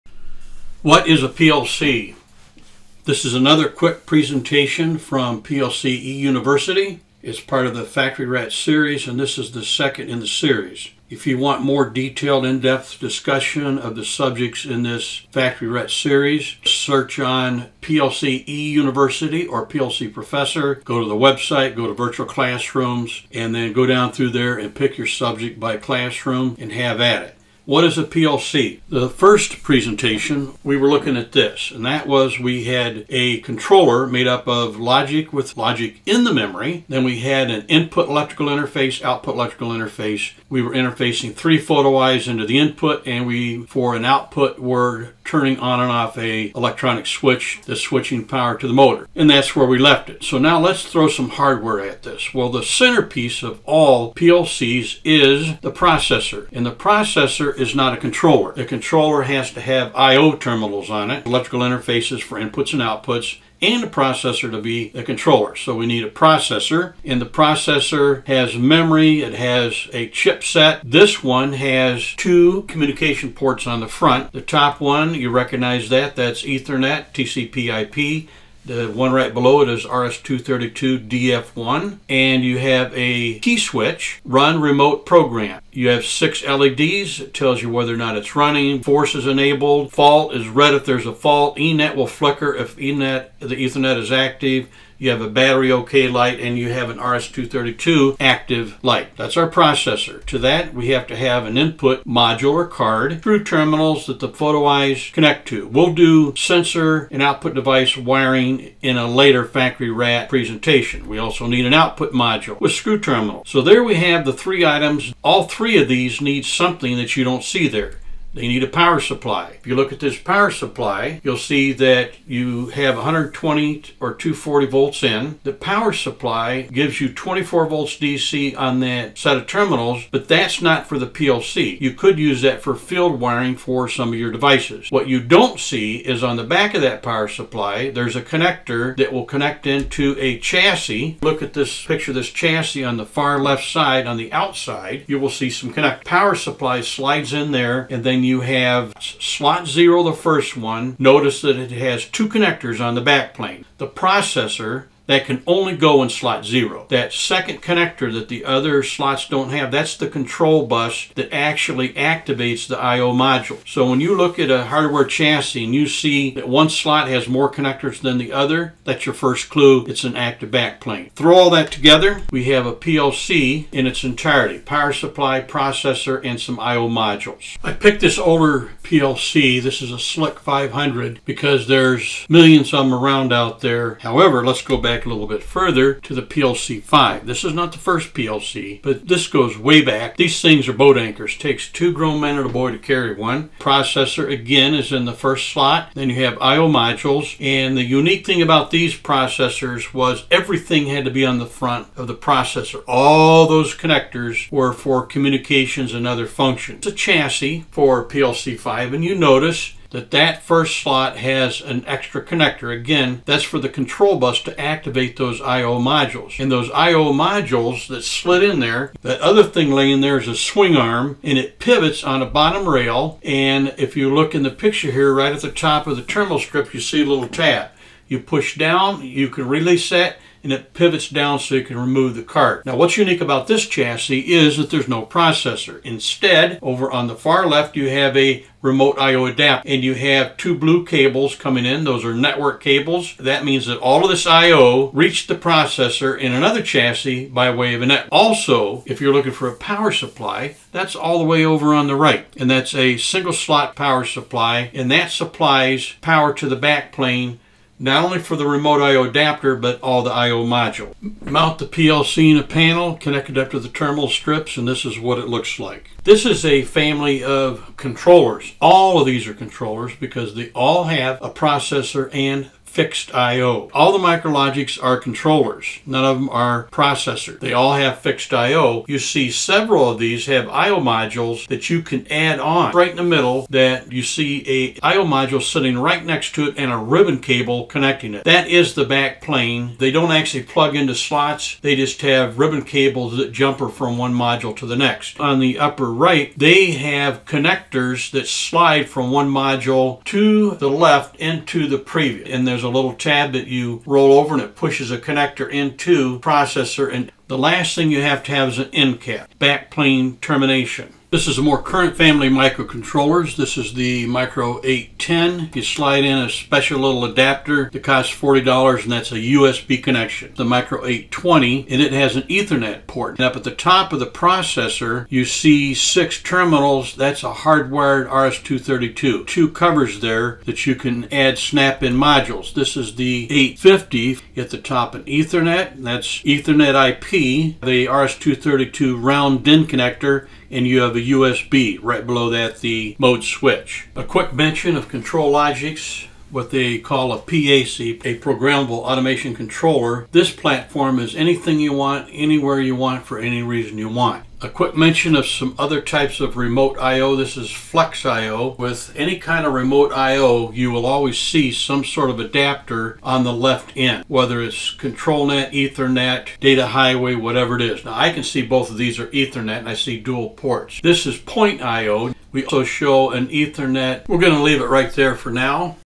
1D - What is a PLC? - Hardware
A series of quick overviews of the elements of a PLC...this one throws some hardware at the subject...really fast with minimum details...more details later.